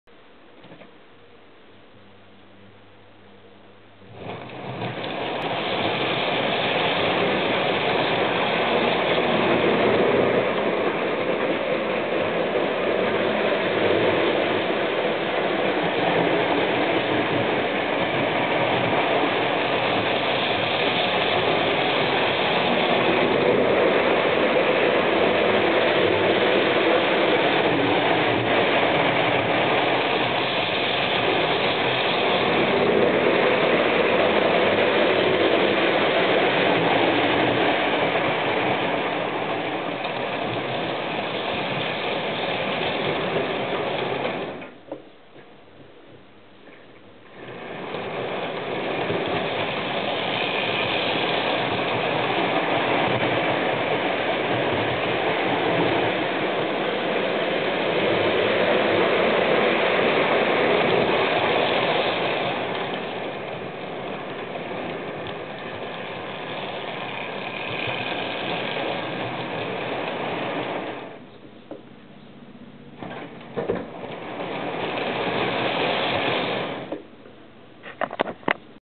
GMR Models LNER Class A4 Merlin 4486 with double tender pulling a rake of LNER Teak coaches
GMR Models LNER Class A4 Merlin 4486 with double tender pulling a rake of LNER Teak coaches for Hornby Dublo 3 rail